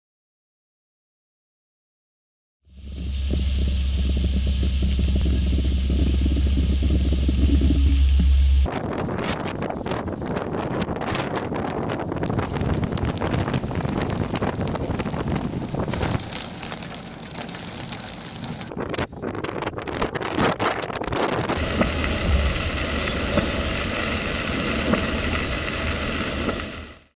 Mau tempo no Minho
Minho em alerta devido a neve, chuva, vento e trovoada.